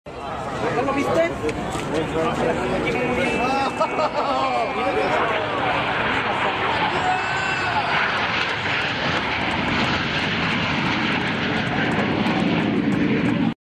Blue Angels Inverted FlyBy
From the 2008 San Antonio air show